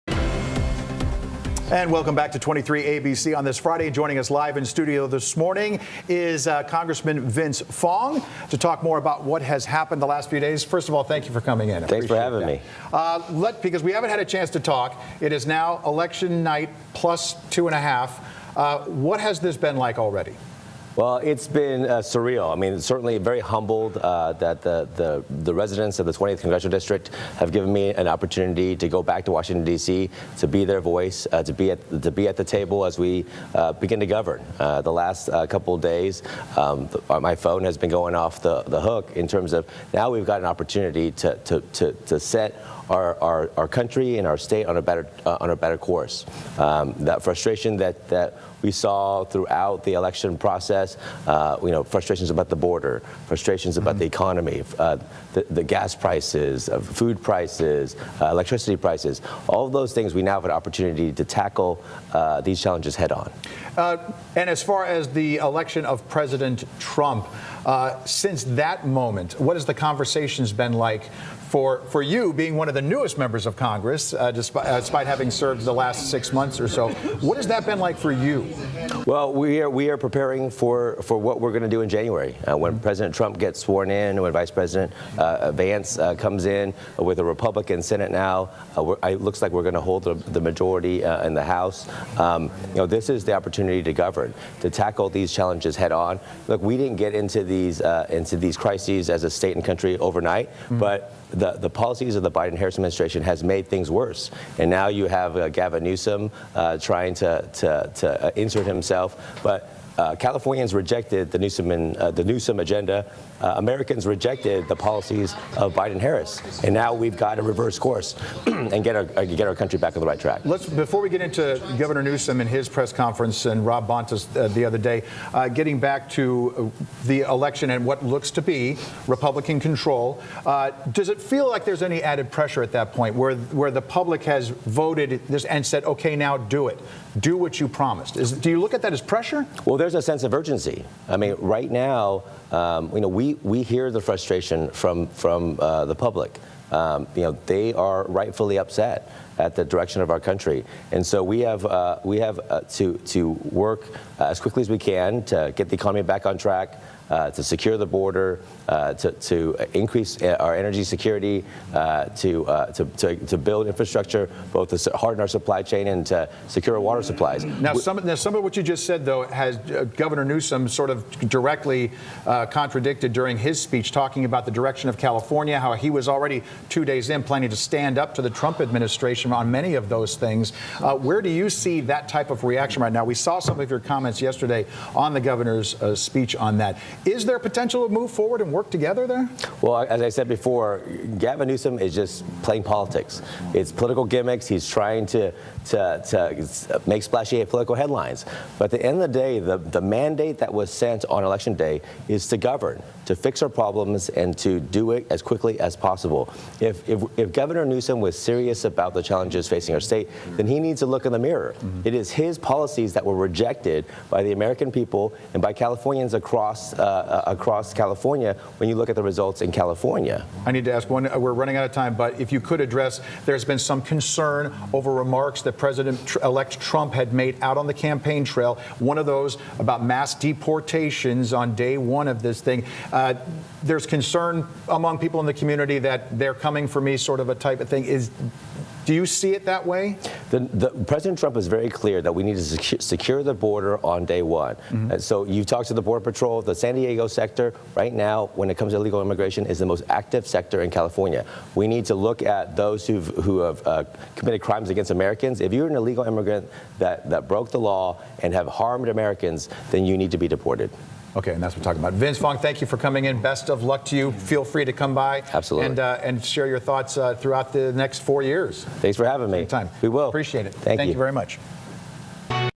Bakersfield Congressman Vince Fong Discusses 2024 Election
Latest Kern County, California and US and world news from 23ABC in Bakersfield, Calif.